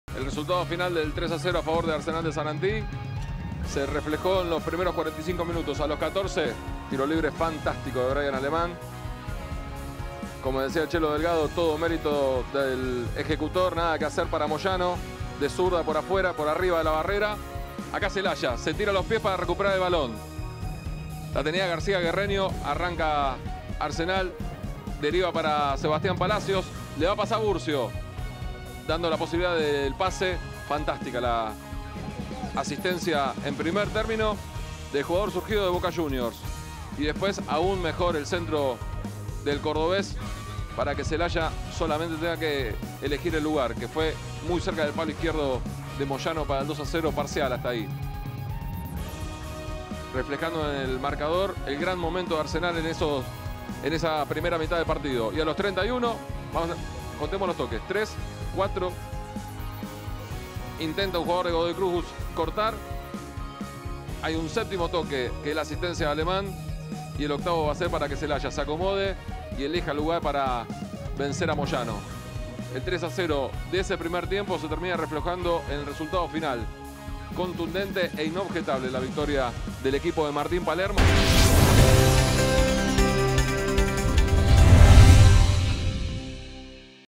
Arsenal hizo la diferencia en el primer tiempo y goleó a Godoy Cruz
Los de Sarandí tuvieron una sólida actuación y definieron el pleito en la primera mitad con goles de Alemán de tiro libre y Zelaya en dos oportunidades. Los mendocinos siguen hundidos en la tabla.

Arsenal goleó a Godoy Cruz de Mendoza por 3-0,  en el partidos jugado en Sarandí, por la novena fecha del torneo de fútbol de Primera División.

Los goles del conjunto que dirige Martín Palermo los convirtieron el uruguayo Brahian Alemán (14m. PT) y Emilio Zelaya (23m. y 31m.) en dos ocasiones.